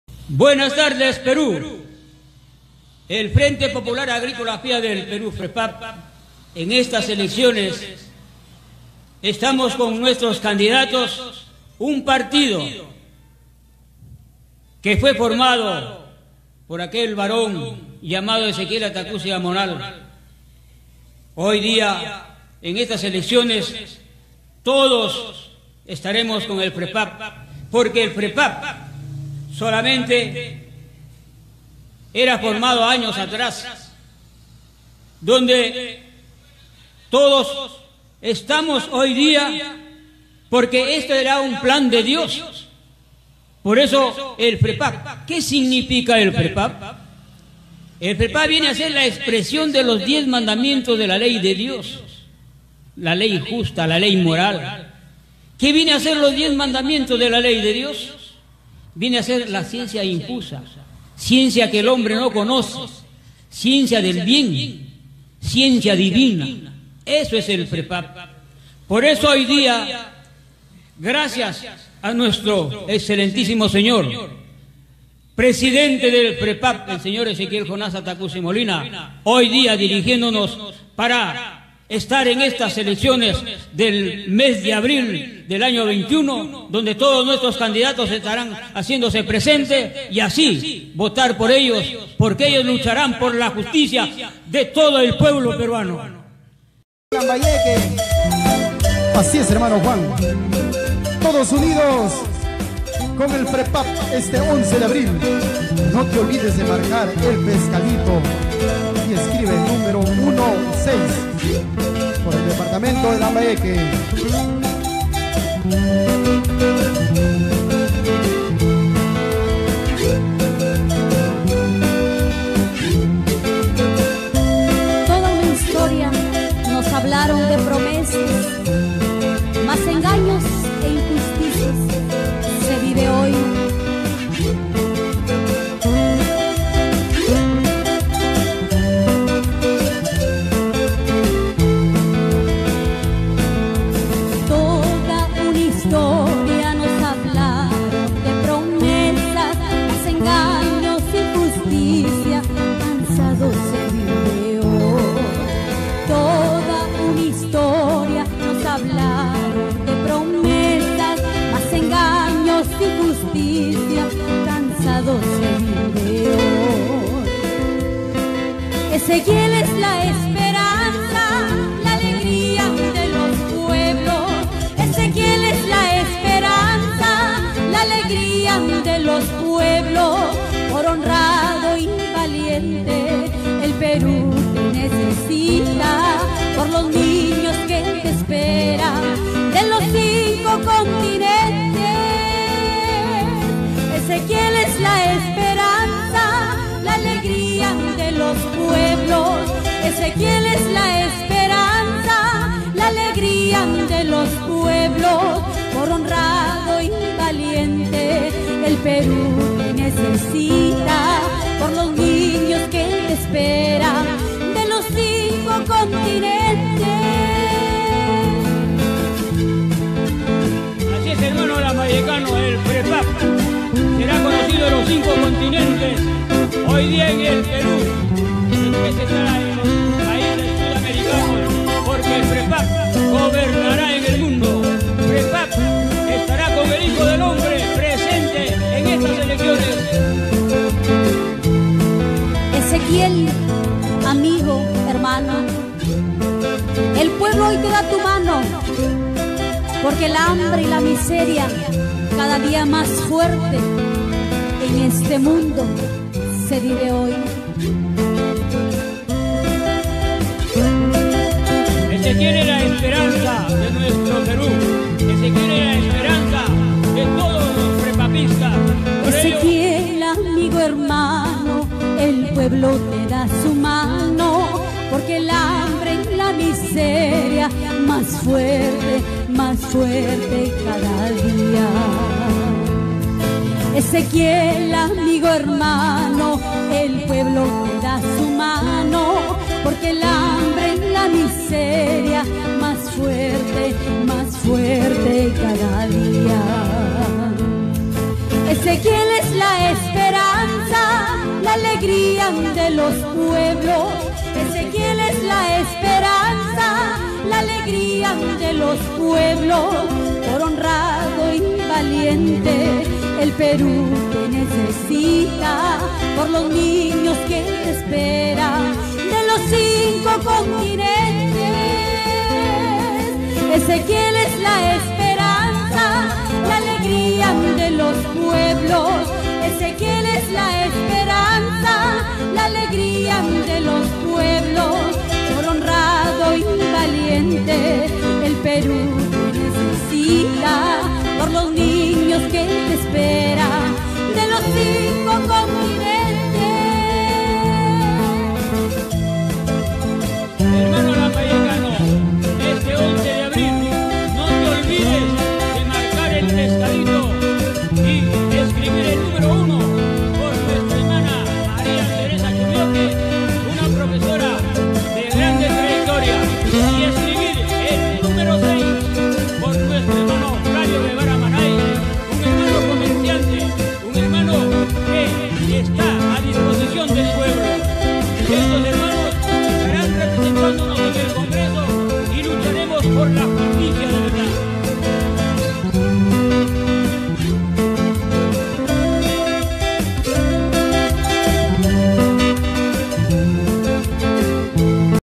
Frepap al congreso 2021 por la región Lambayeque
María Teresa Chumioque Castañeda al congreso/ Nº1 chiclayo_Perú

Presentamos a nuestro candidato del Frepap por el nro 6 en el departamento de Lambayeque todossomosfrepap 🐟💙💪😀